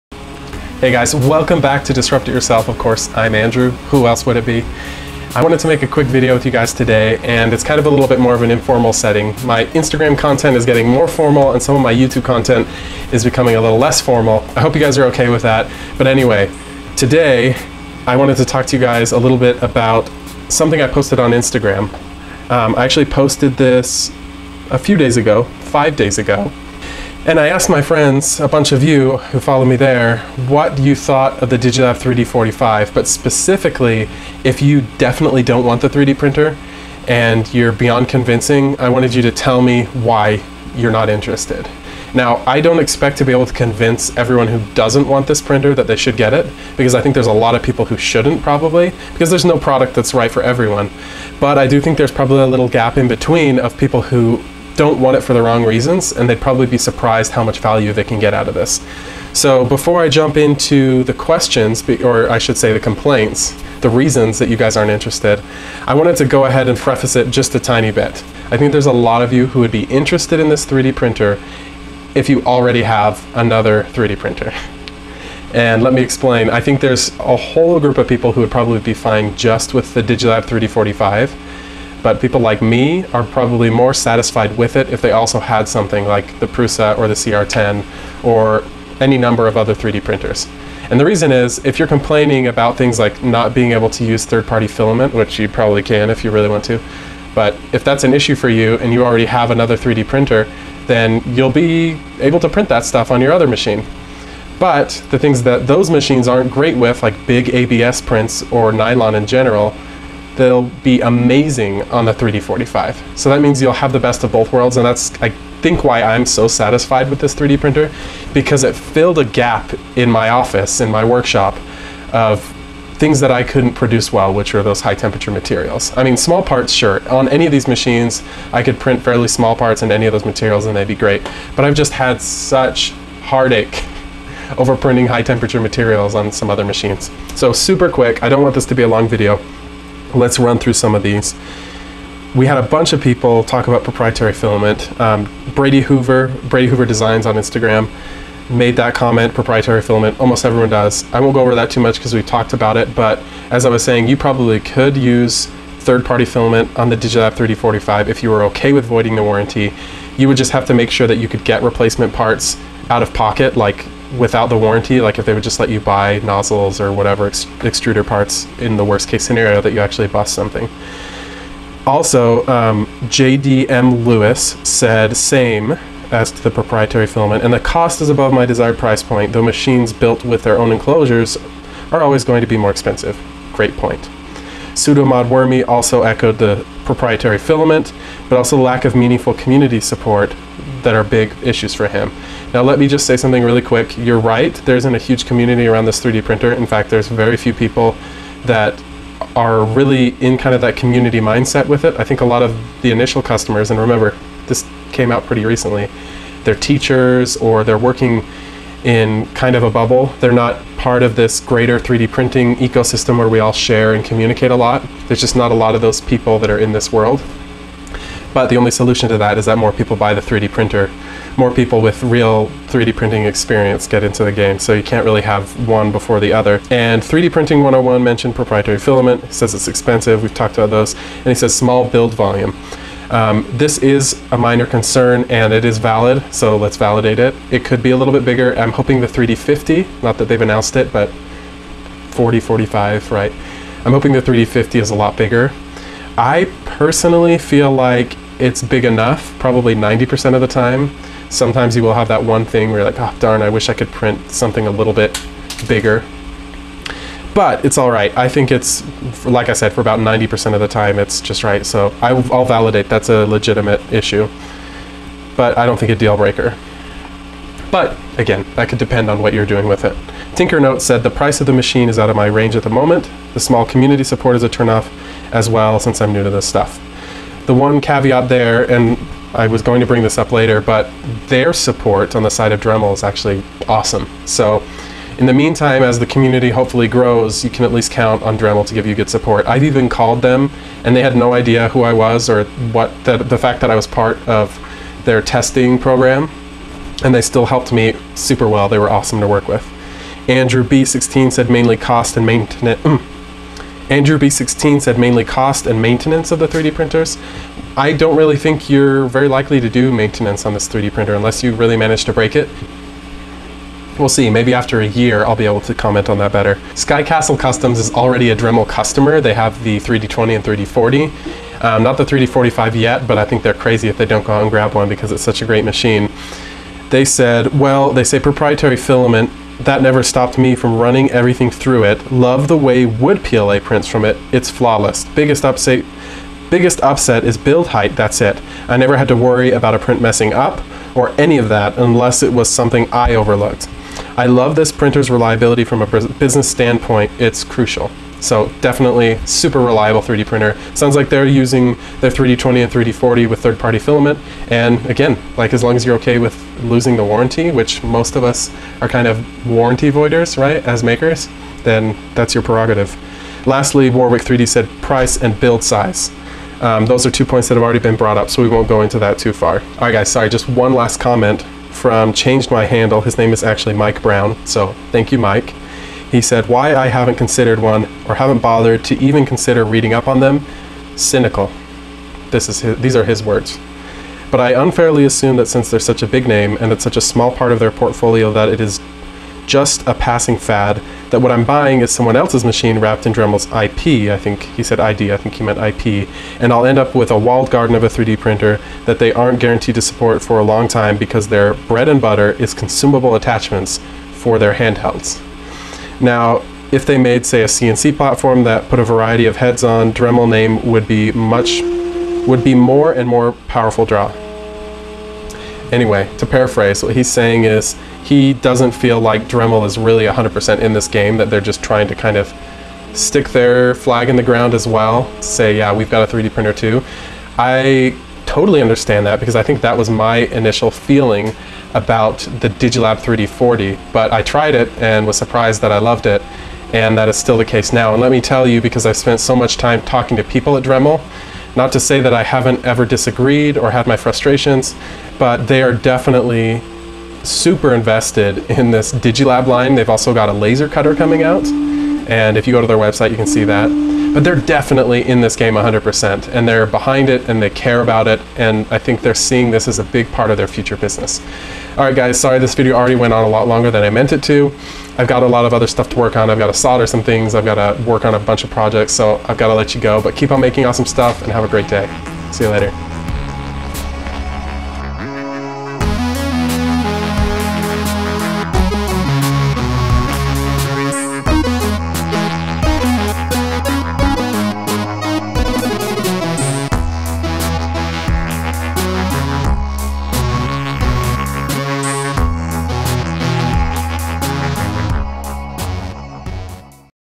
DigiLab 3D45 Q&A
Pretty simple. Made a video to address the main concerns my Instagram Followers have with the Dremel DigiLab 3D45.

Thank you so much to everyone who took the time to answer my questions. This was the original post: